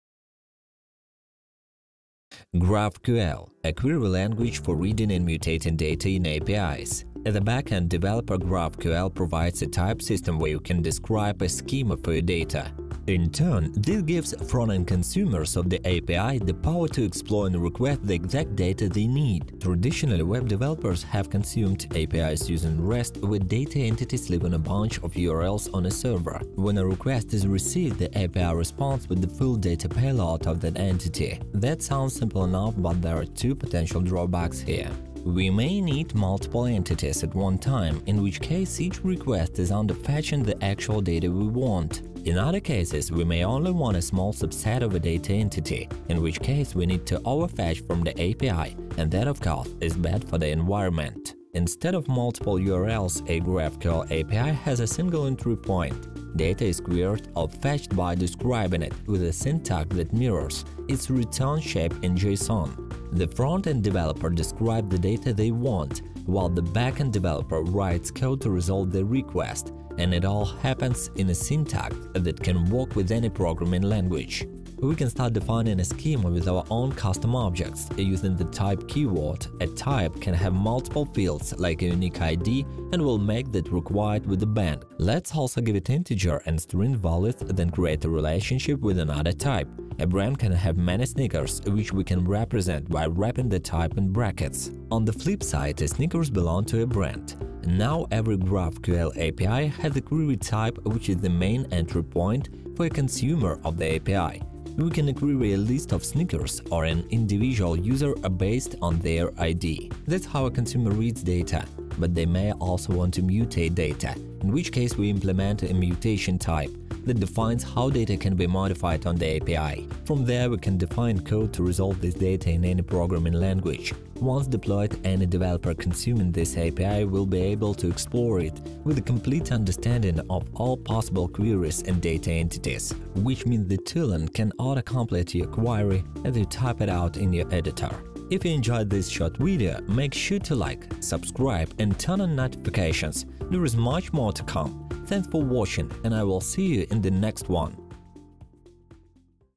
GraphQL Tutorial in 2 Minutes 📚
What is GraphQL?

In this quick 2-minute video, you'll learn the basics of GraphQL and how it compares to REST APIs.

GraphQL is a query language that enables developers to retrieve the exact data they need from an API, rather than over-fetching or under-fetching data, while REST API requires multiple requests to retrieve the same data.

In this tutorial, you'll learn how to define a schema, create queries, and make requests to a GraphQL API.

Whether you're a beginner or just need a refresher, this tutorial will give you a solid understanding of GraphQL and how it compares to REST API in just two minutes.

Get ready to level up your API development skills!